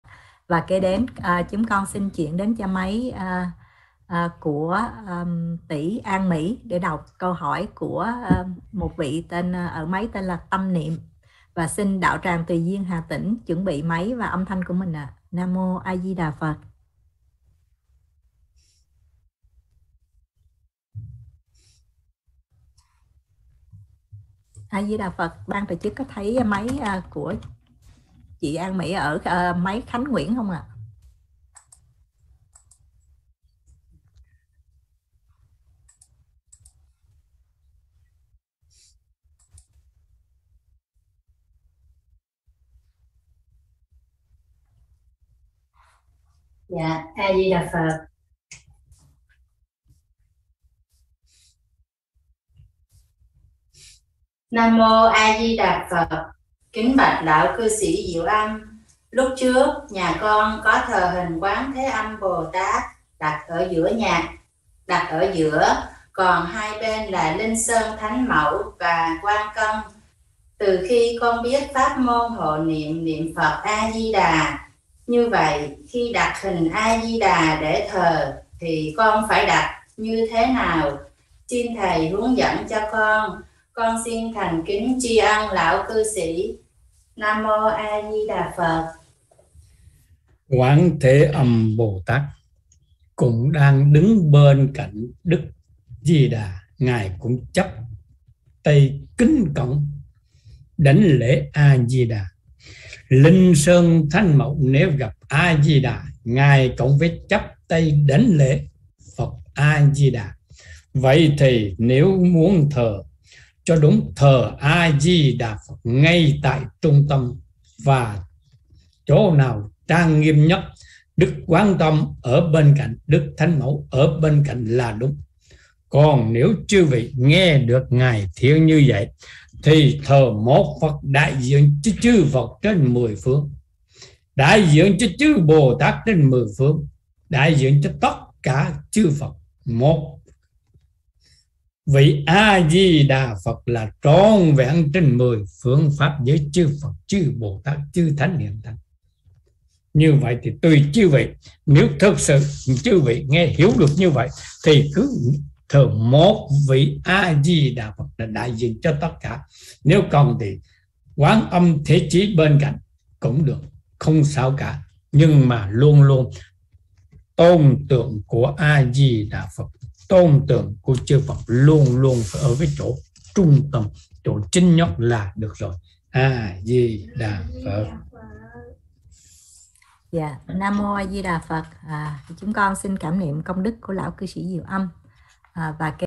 Thờ Phật A Di Đà Phải Đặt Ở Vị Trí Như Thế Nào Cho Đúng? Lão Cư Sĩ Diệu Âm
Câu hỏi: Trước Đây Có Thờ Ngài Quan Âm Bồ Tát Ở Giữa Còn Hai Bên Là
Linh Sơn Thánh Mẫu Và Quan Công  Nhưng Từ Khi Biết Đến Pháp Môn Niệm Phật - Hộ Niệm - Vãng Sanh Thì Phải Đặt Đức Phật A Di Đà  Ở Vị Trí Như Thế Nào Cho Đúng?